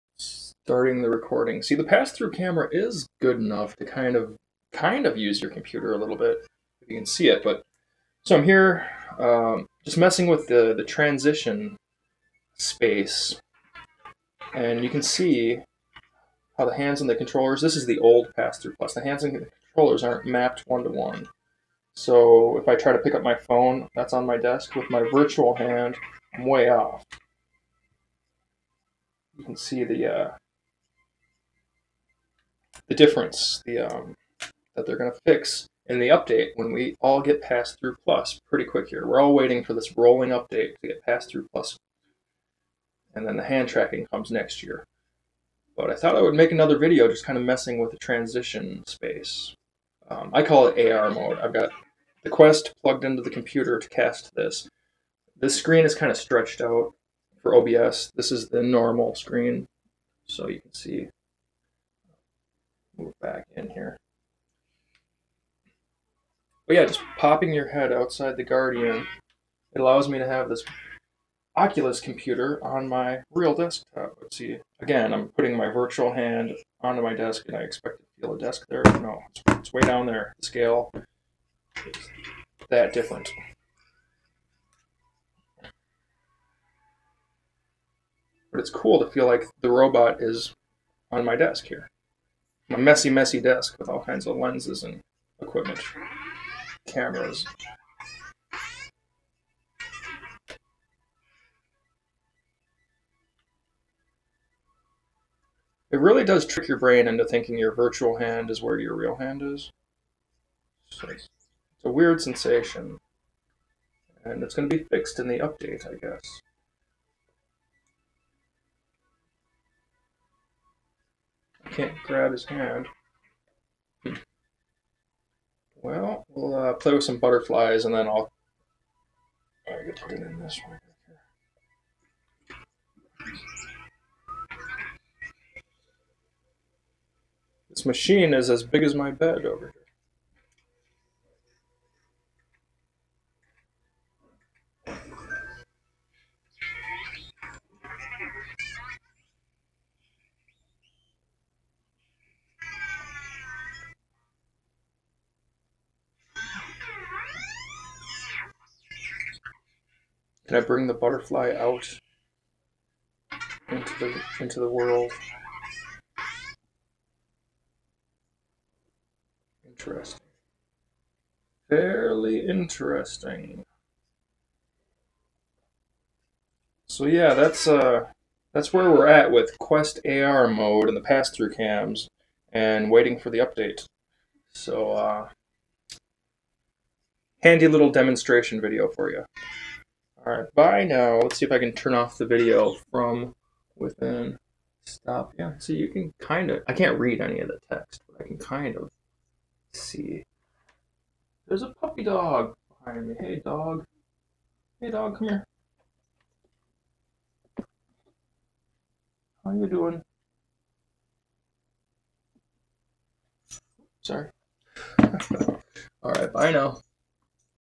Last Look at OculusQuest Pass Through Camera System (Before Update 9)
One last look at the way it used to be in Oculus Quest Pass Through Camera mode, before the 9.0 update rolls out. Oculus VR is cool, but when you step outside the guardian, you can experience a begining vision of an AR mode in the transition space. Only, the pass through video is not exactly synced up with stereo correct virtual world objects. Size and distance are slightly askew. This visual dynamic will change in update 9.0, adding some very efficient computer vision processing to the pass through video. We will be getting "Pass Through Plus", like the Rift S has. Then the next phase will be controller-less hand tracking. So, I'm just documenting the consumer experience before things change.